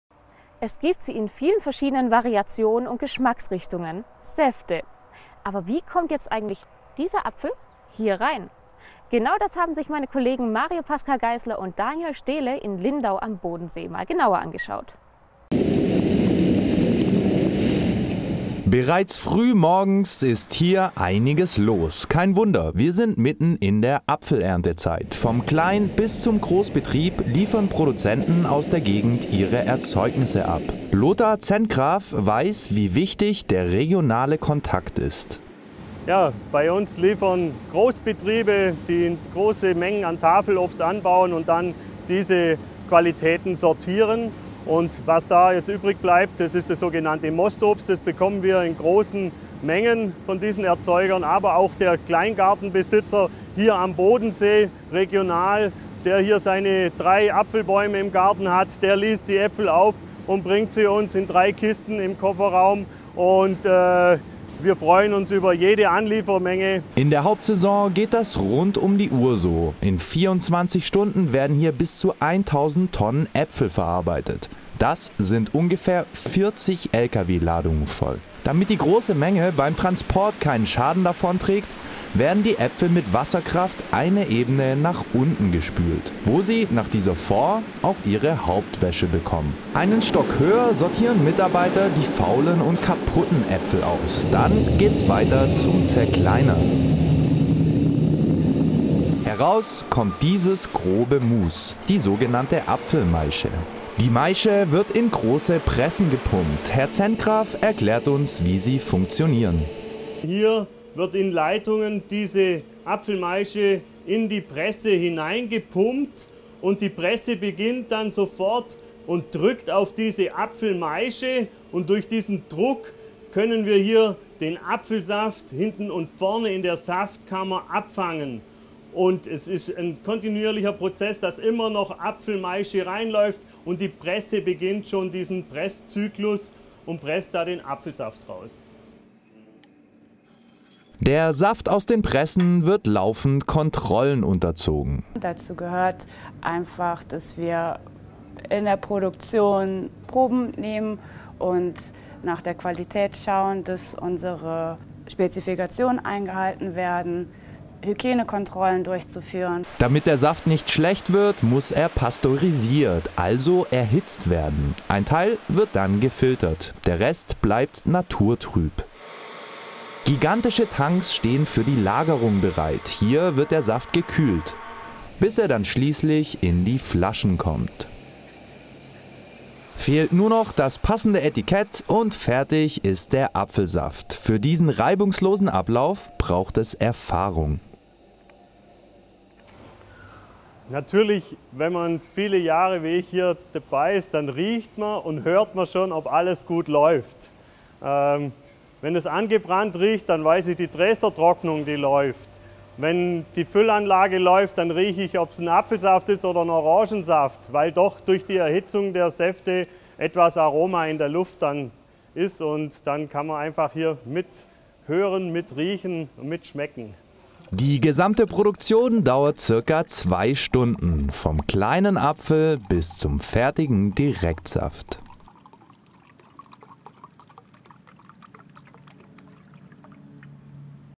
Apfelsaft vom Bodensee
Vom Baum ins Glas: so wird aus Äpfeln frischer Saft.